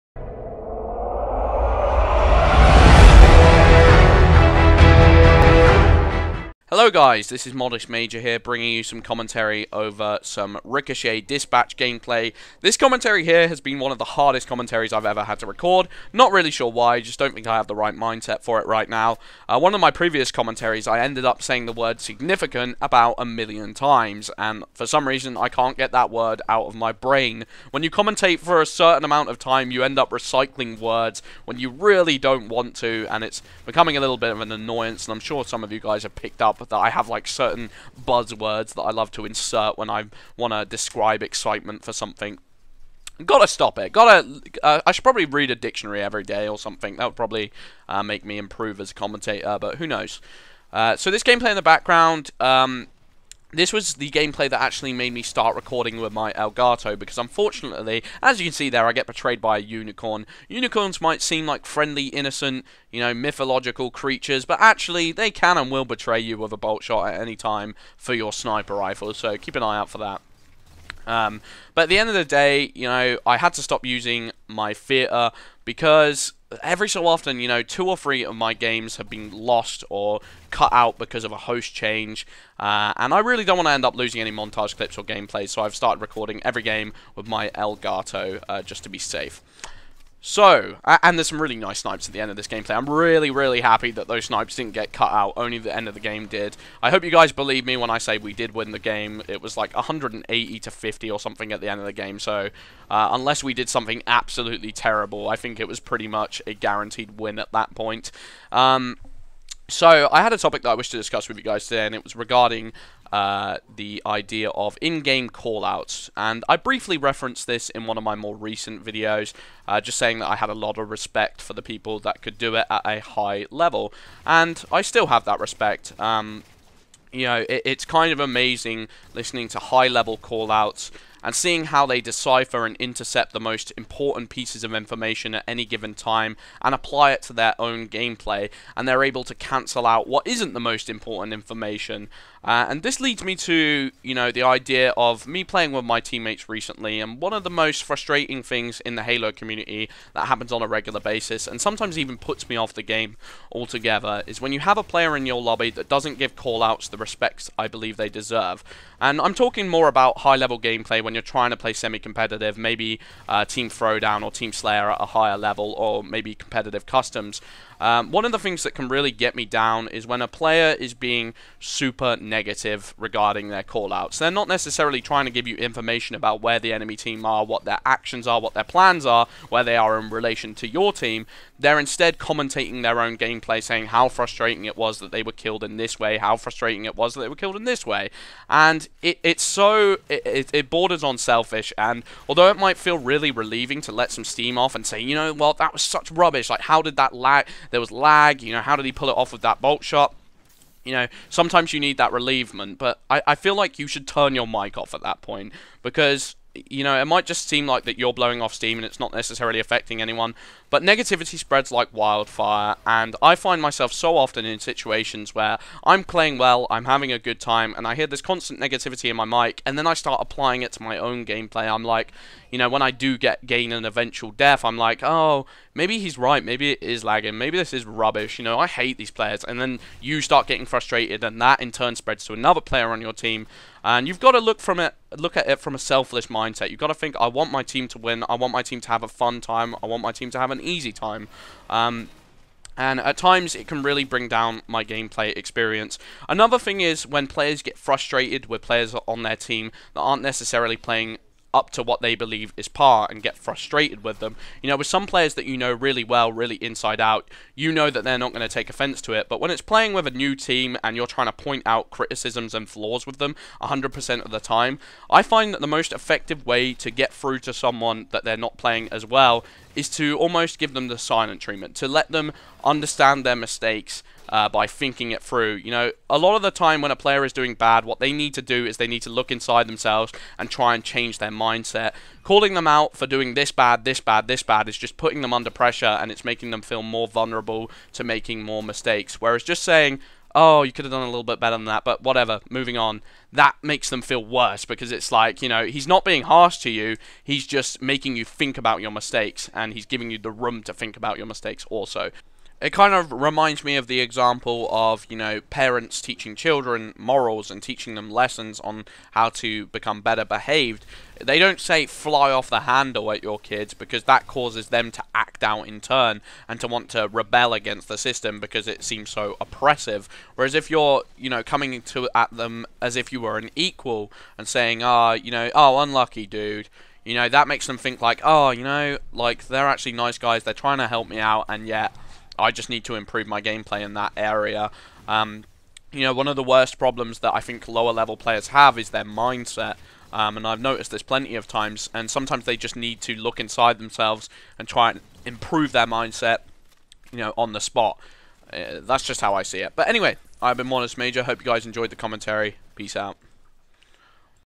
Ricochet: Communication Breakdown
Today I talk about communication and how it can have a big effect on the success rate of your team.